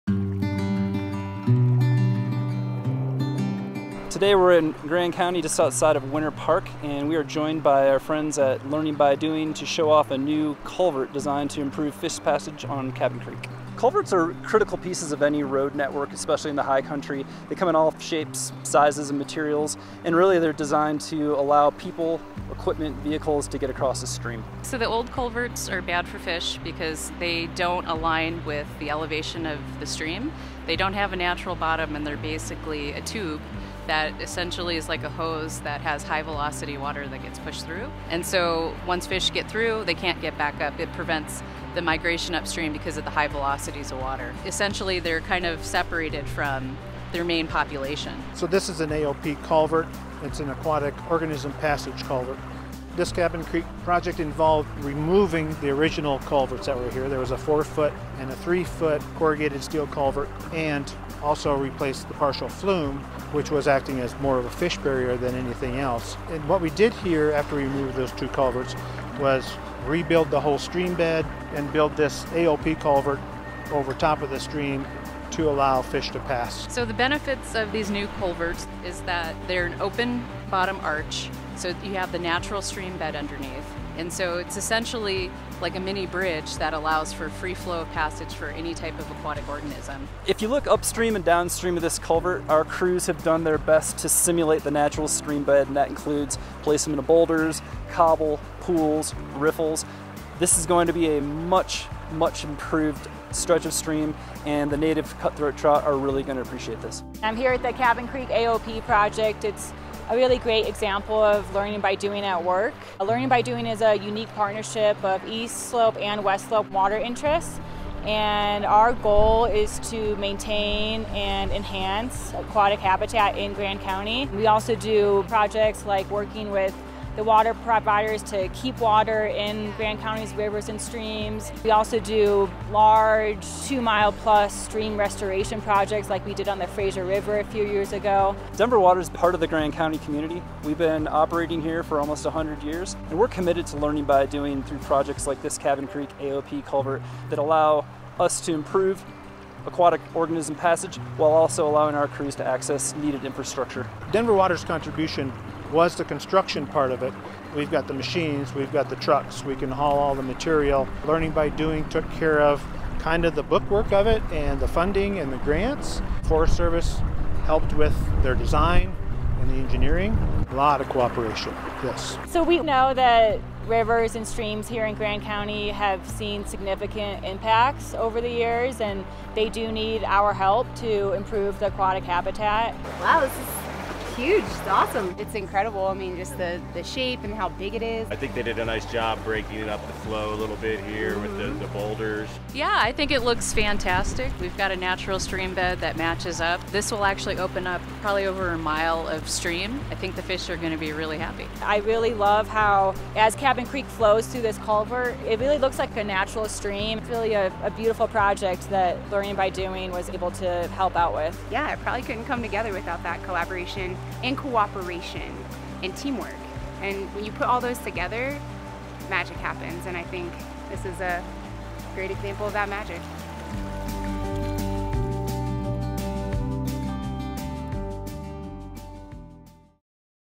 New aquatic organism passage on Cabin Creek
In the fall of 2021, Grand County Learning By Doing partners worked together to install a new aquaticorganismpassage on Cabin Creek. Learn how the structures help the Colorado River cutthroat trout.